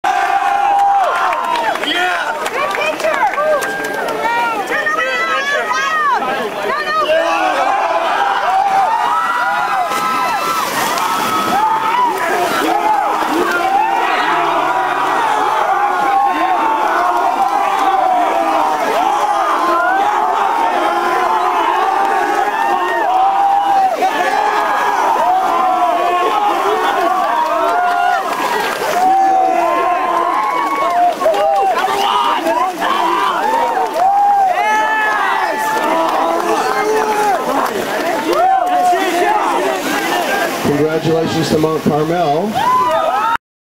MCHS Mens CIF champs dunk coach Peck
Mount Carmel High School Mens Swim Team captures 2007 CIF crown, and celebrate in style.